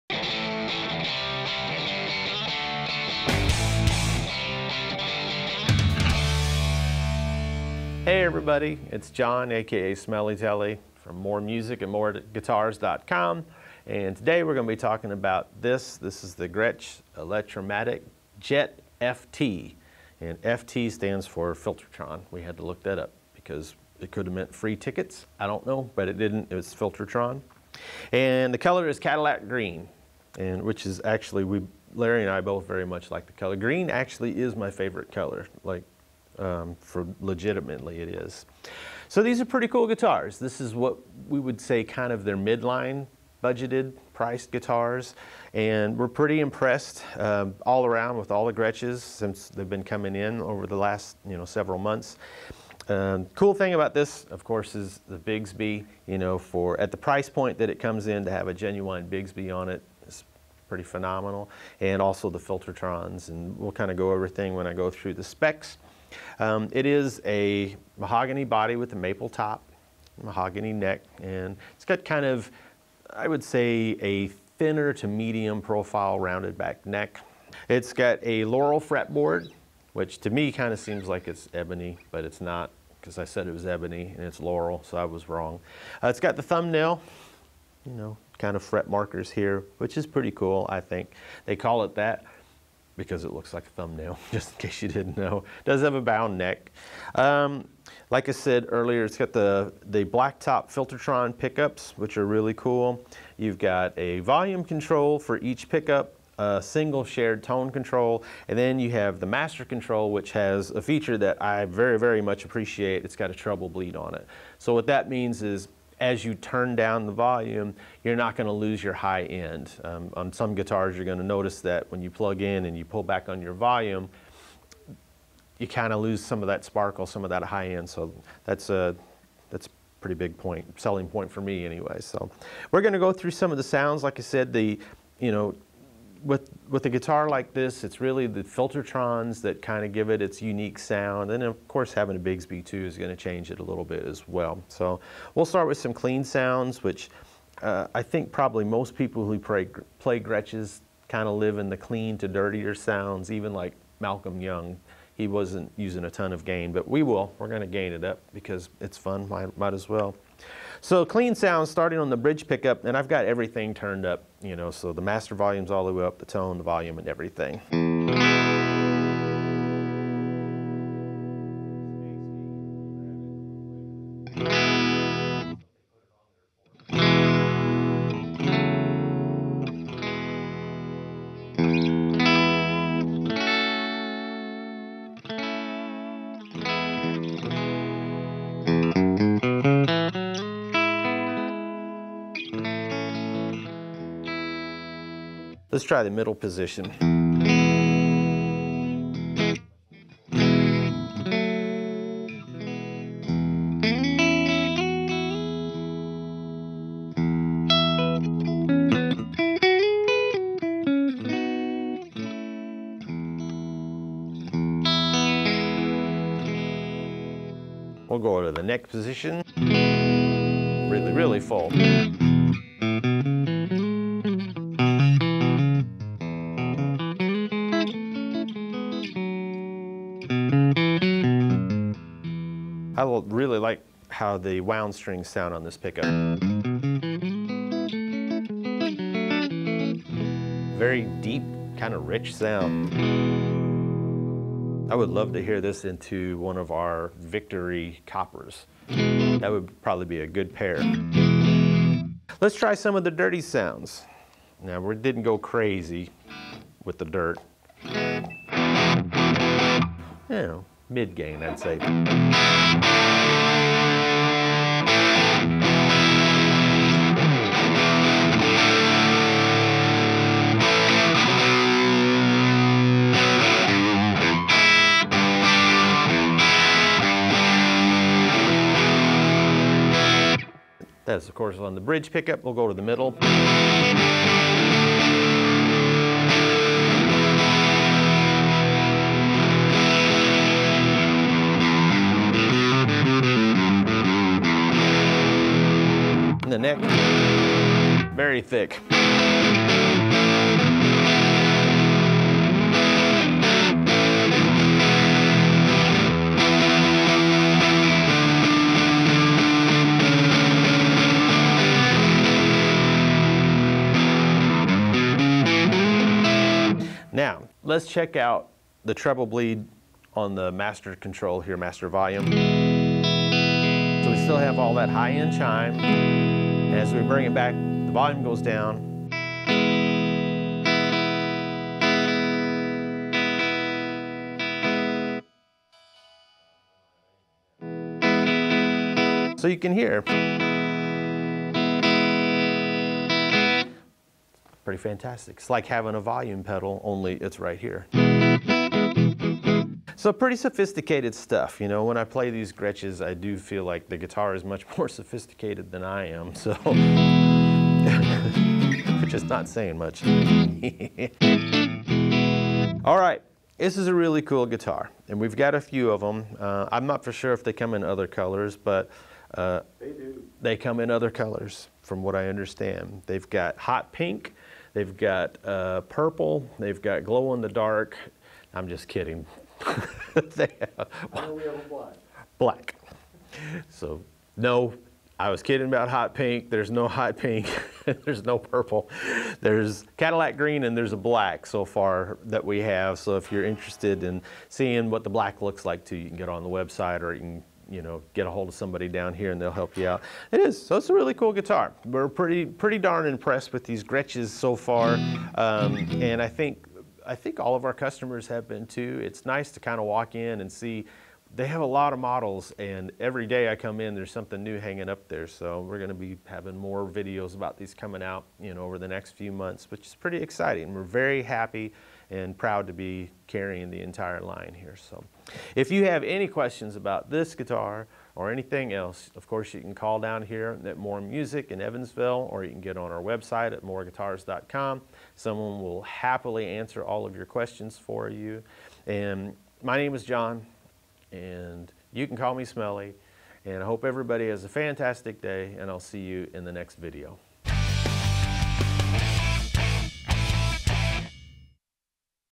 Gretsch G5230T Electromatic Jet FT {Bigsby on a budget!}
The Gretsch Electromatic Jet is another great playing and sounding. yet affordable Gretsch offering, with some surprising features for the price. Filter'Tron pickups, a B50 Bigsby trem, bound fretboard, and Gretsch's Master Volume with treble bleed circuit.
The Gretsch line continues to pleasantly surprise us with each new guitar we unbox. They're great playing guitars with unique, modern tones.